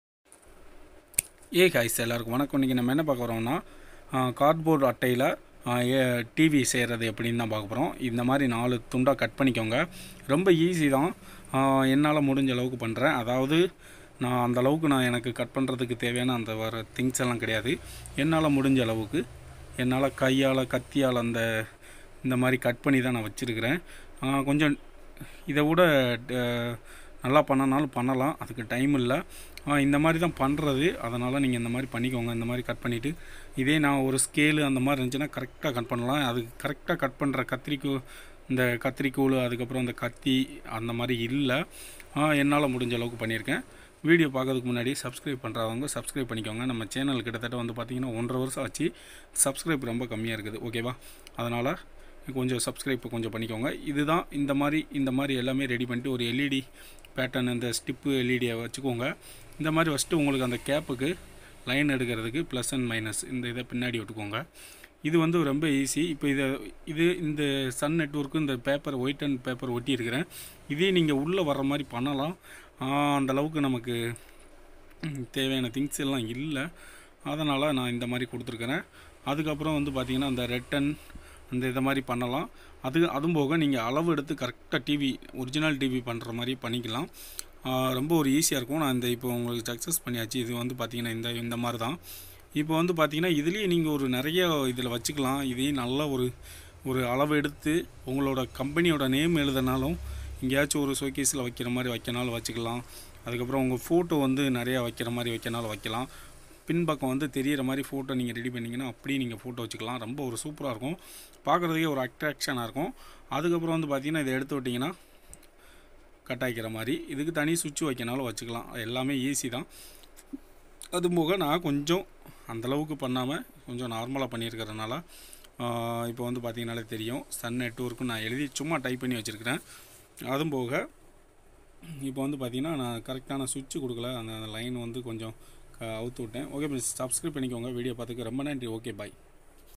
How To Make LED TV at home Using Cardboard - Making Cardbord Tv | Mahendra Tech
Mahendra Tech